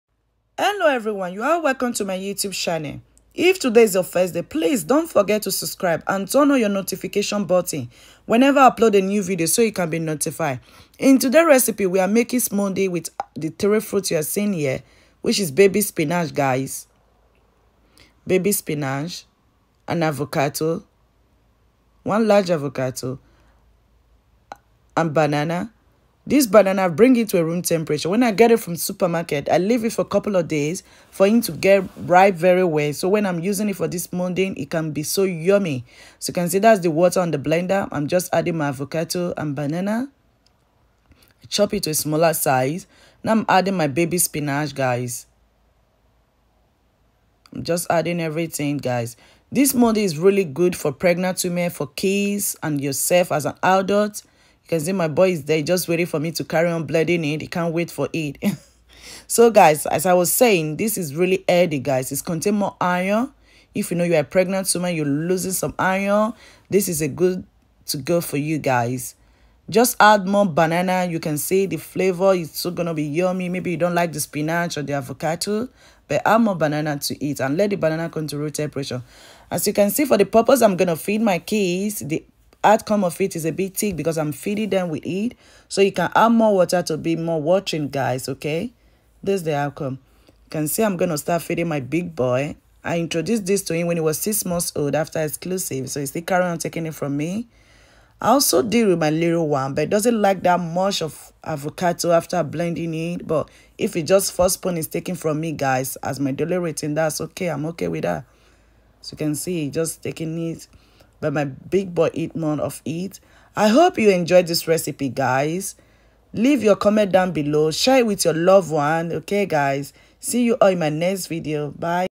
HOW TO MAKE HEALTHY SMOOTHIES | spinach and Avvocato￼
Recipe

This smoothie is good for adult children and pregnant, is very rich in iron If you want your kids to take it you need to leave the banana in the room temperature for like four days to become very ripe and so yummy so it is good to go for your blender smoothie￼

Baby spinach￼ 60g
Small avocado￼ 1
Banana￼   2